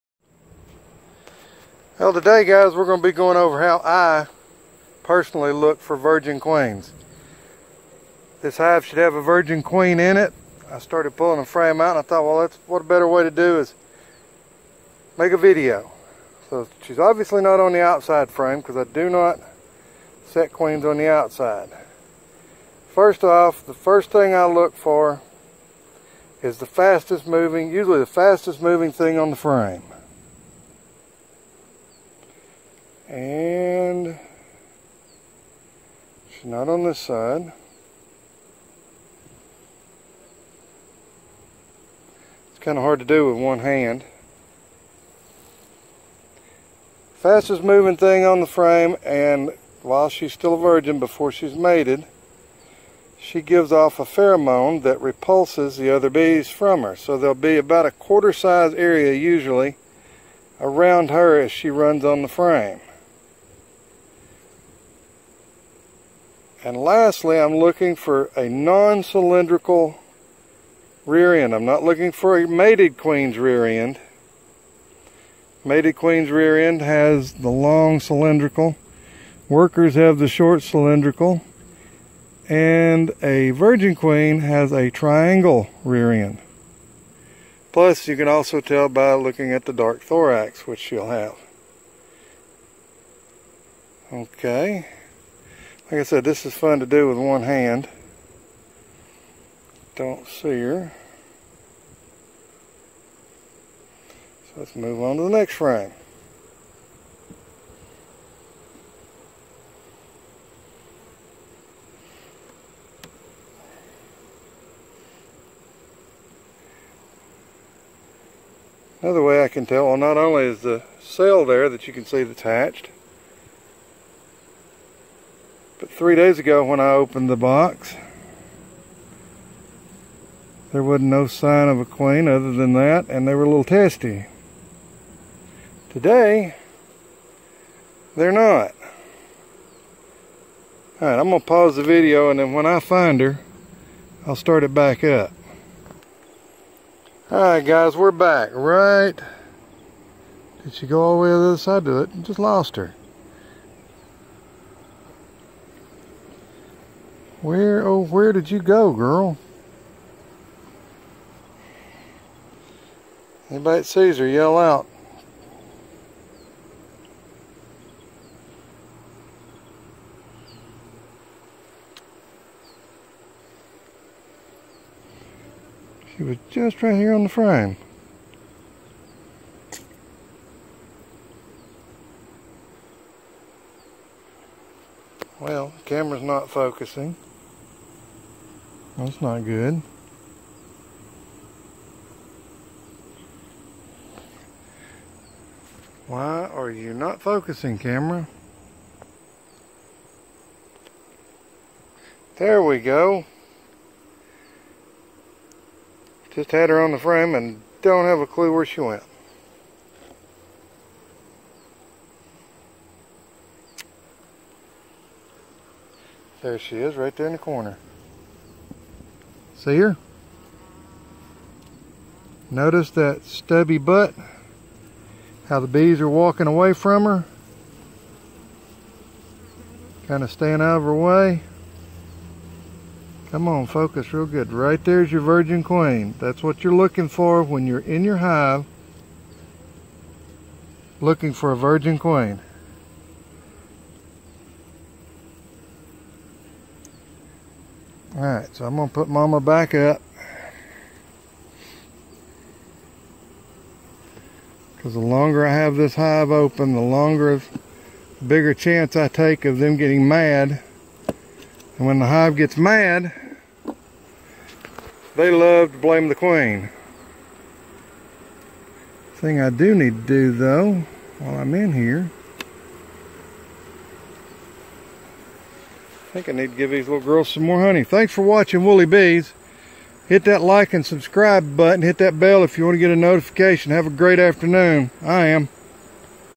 How to Find a Virgin Honey Bee Queen in Your Hive!
We look at what i look for when trying to find a Virgin Honey Bee Queen in my hives.